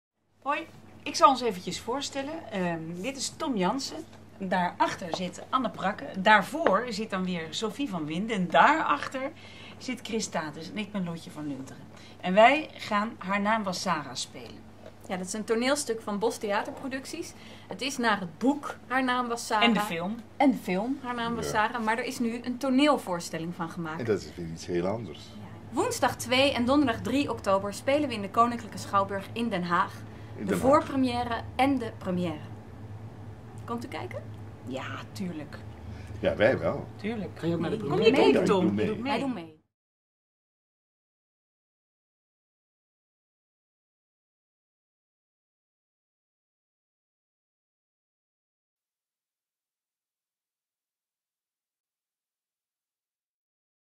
Uitnodiging Haar naam was Sarah KS Den Haag
De cast van Haar naam was Sarah nodigt u uit op woensdag 2 en donderdag 3 oktober in de Koninklijke Schouwburg Den Haag.

Aangrijpende roman Haar naam was Sarah voor het eerst op het toneel

Regisseur Ursul de Geer en bewerker Léon van der Sanden brengen vanaf september het toneelstuk Haar naam was Sarah naar het theater, met in de hoofdrollen Lotje van Lunteren (Bernhard, Vastgoedfraude), Sophie van Winden (Tirza, Overspel), Tom Jansen (Penoza, Ramses), Chris Tates (Liefde Levenslang, De Eetclub) en Anne Prakke (De Avonden, Win/Win). Dit gouden duo maakte eerder al, ook in samenwerking met Bos Theaterproducties, het geprezen stuk De Aanslag van Harry Mulisch (finalist Toneel Publieksprijs 2011) en Napoleon op St. Helena (met Huub Stapel).

Haar naam was Sarah gaat over een zoektocht naar het verleden, waarbij herkenbare thema's als liefde en loslaten aan bod komen. De Amerikaanse journaliste Julia doet onderzoek naar de razzia van 16 juli 1942 in Parijs, een zwarte bladzijde uit de Franse geschiedenis. In haar zoektocht stuit Julia op het verhaal van Sarah, een 10-jarig meisje dat aan de razzia is ontsnapt. Hierdoor ontdekt zij per toeval een verschrikkelijk geheim dat haar schoonfamilie voor haar verborgen wilde houden. Ondanks herhaaldelijke verzoeken van de familie om te stoppen met het graven in het verleden wil Julia koste wat kost de onderste steen boven krijgen. Door haar vastberadenheid dreigt Julia de grip op haar eigen leven te verliezen, met grote gevolgen voor haar huwelijk.

Gebaseerd op de roman Haar naam was Sarah van Tatiana de Rosnay, gepubliceerd in Frankrijk door Editions Héloïse d'Ormesson en in Nederland door Artemis.

CREDITS
Naar de roman van: Tatiana de Rosnay
Regie: Ursul de Geer
Bewerking: Léon van der Sanden
Spel: Lotje van Lunteren, Sophie van Winden, Tom Jansen, Chris Tates en Anne Prakke
Scenografie: Herbert Janse
Digitale scenografie: Harry De Neve
Kostuumontwerp: Dorien de Jonge
Lichtontwerp: Uri Rapaport
Scriptdramaturgie: Judith Wendel
Casting: Kemna Casting, Marc van Bree